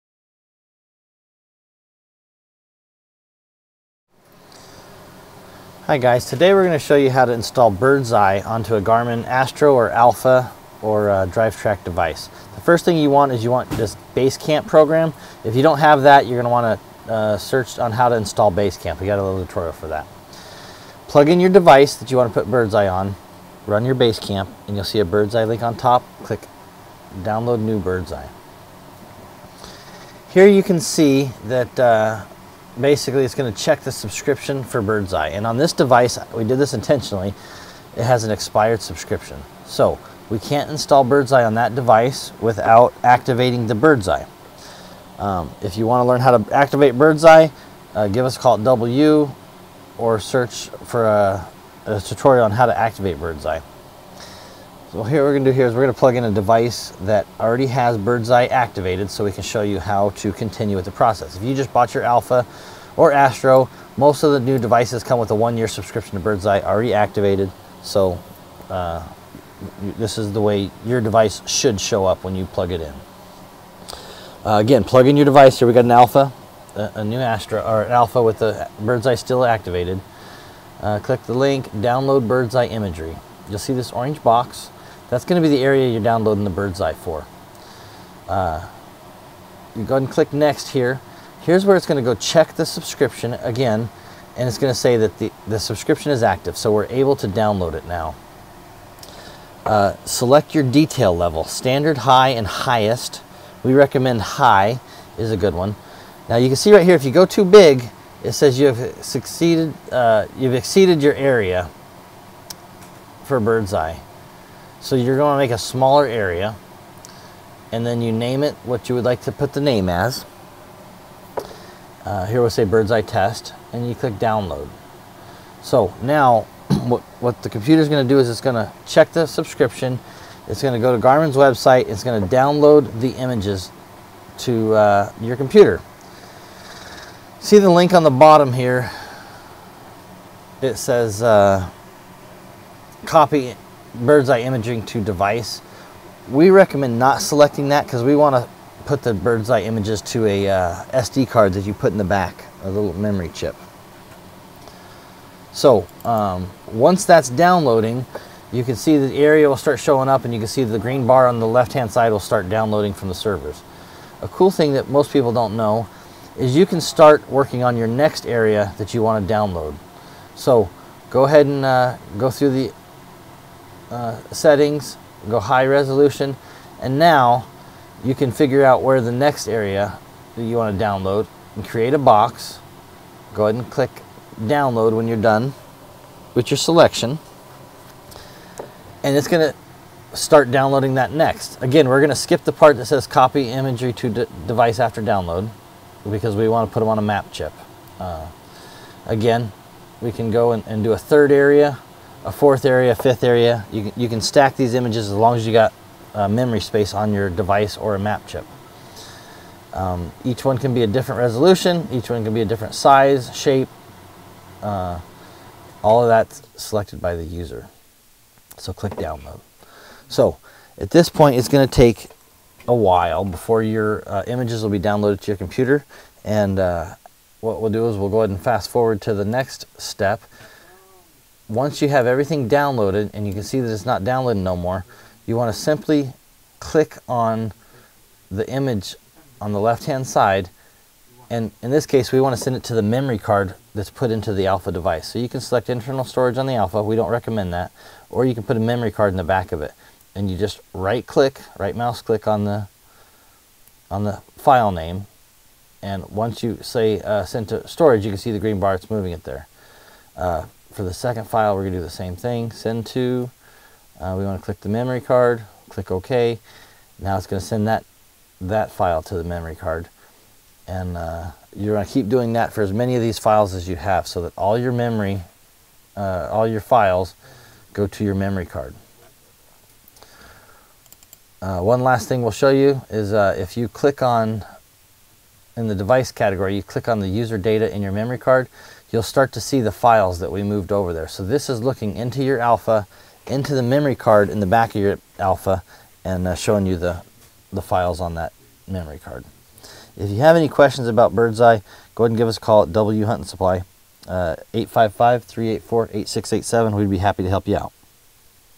Downloading Garmin Birdseye for your GPS Device
Buddy walks you through downloading Garmin Birdeye for your GPS device on the Garmin Basecamp program.